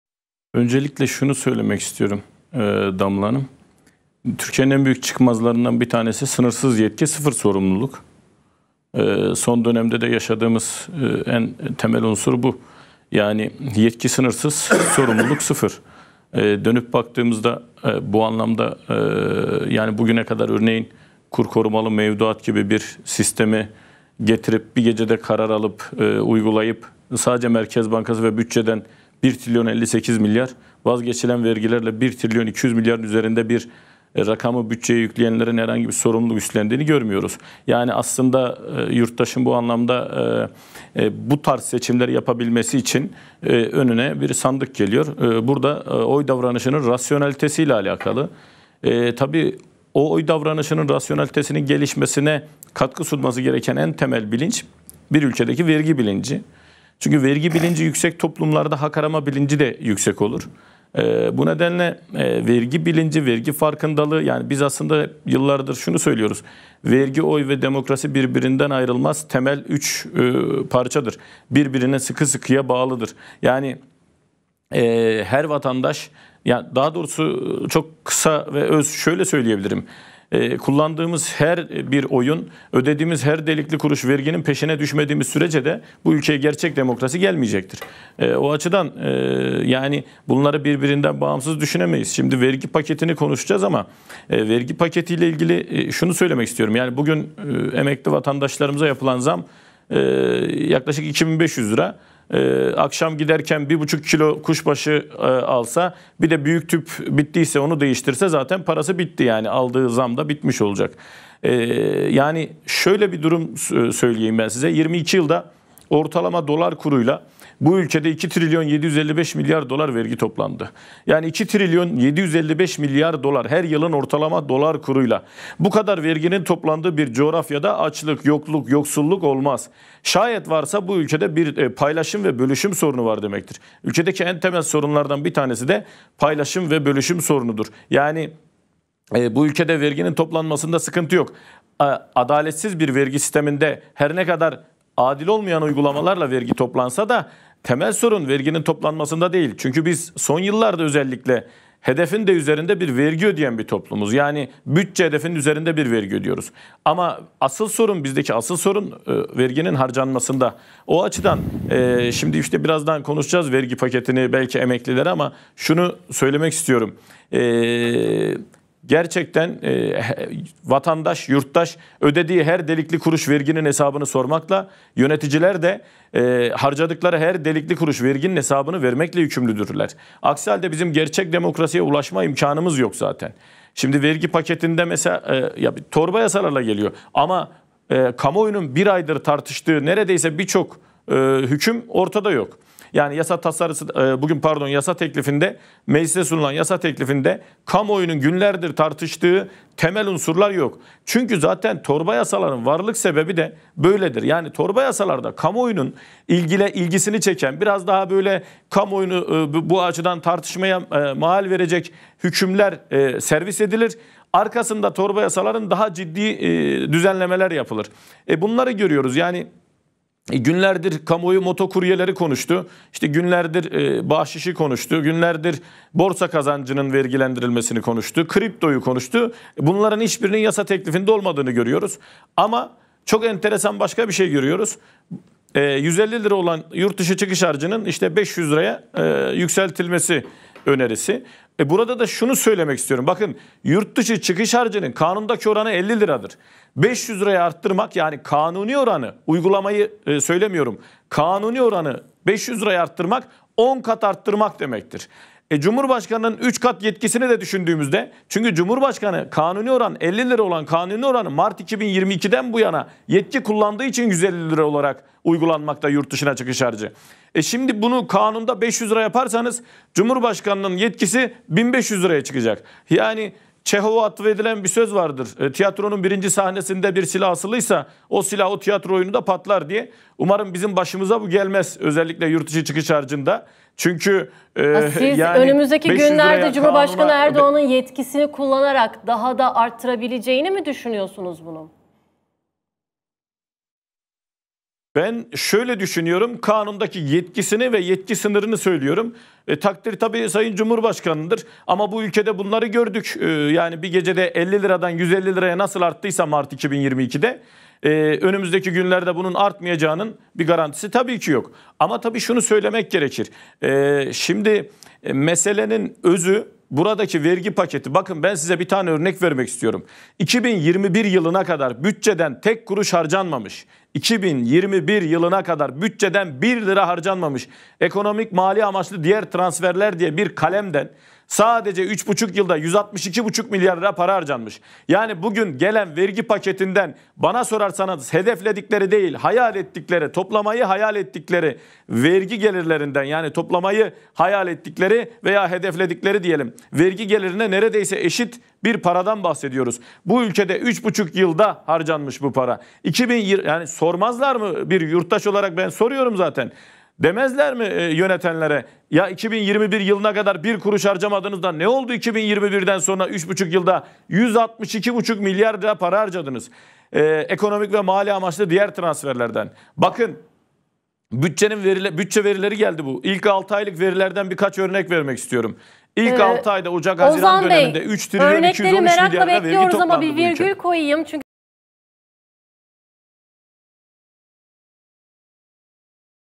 Vergiden Belimiz Bükülecek! Erdoğan O Yetkiyi Kullanırsa Hepimiz Yandık! Ozan Bingöl Anlattı
Digiturk: 60
D-smart: 93
Turkcell TV Plus: 69
Tivibu: 69
Türksat Kablo TV: 76